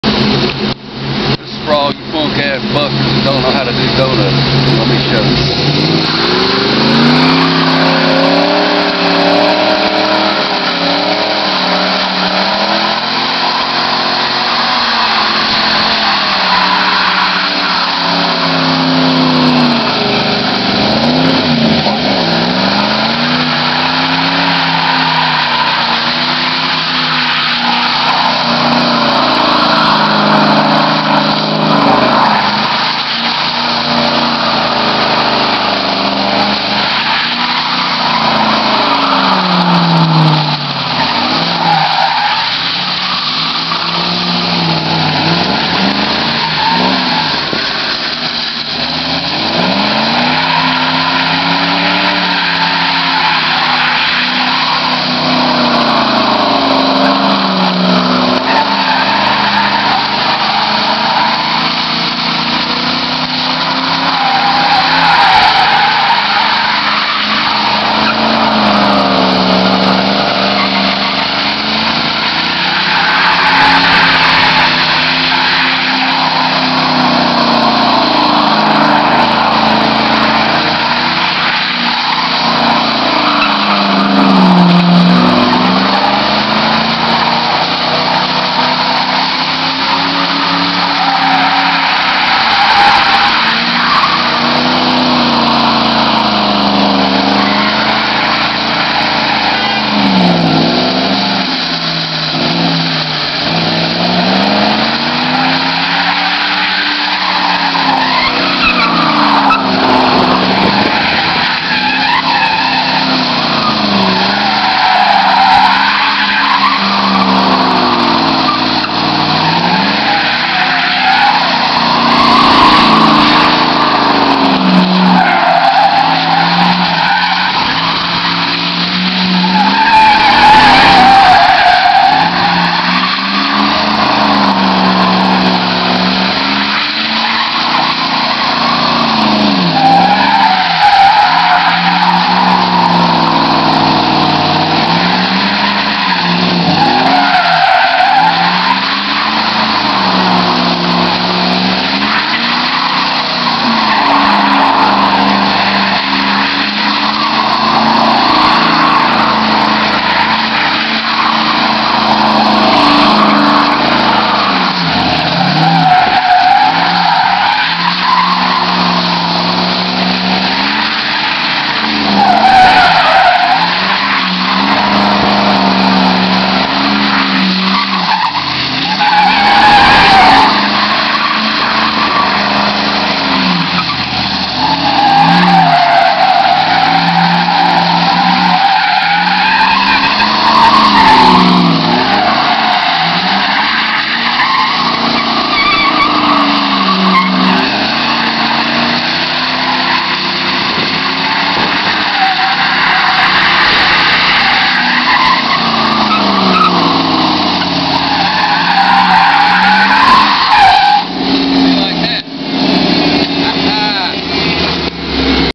Mustang Fun
I was just tired of see all these lame azz burnout and drifting videos = so I made one. please rate and hate...